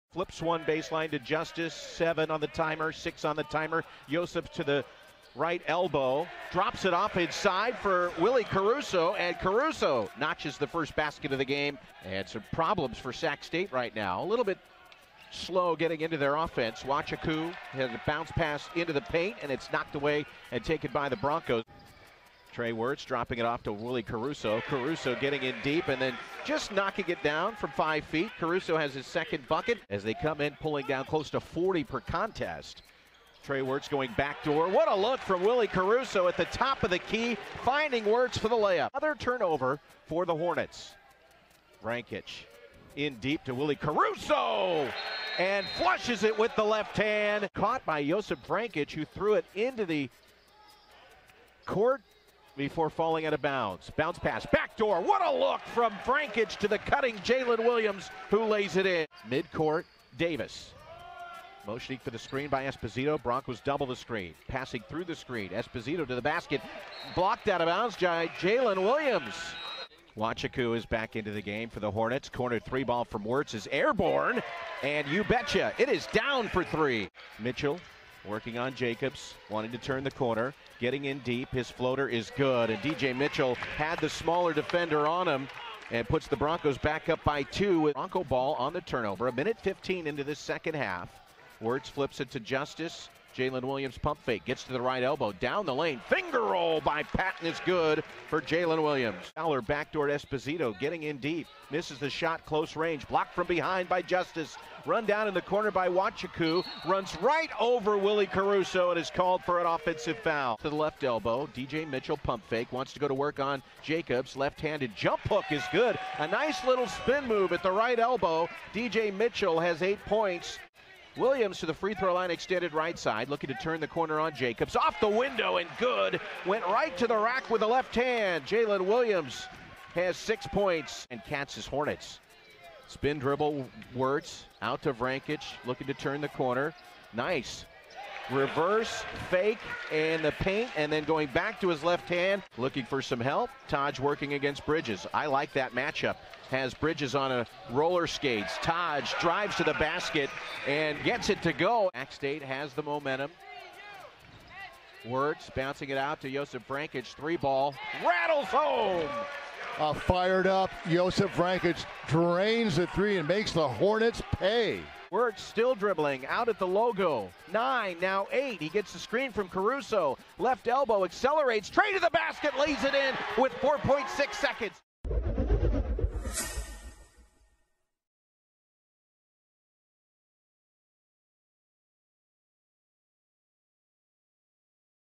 Santa Clara Basketball Top Plays vs. Sacramento State (2019-20) | Stadium
Santa Clara had four players reach double digits in scoring, led by Josip Vrankic's 15 and Trey Wertz's 11, as the Broncos downed Sacramento State 60-58.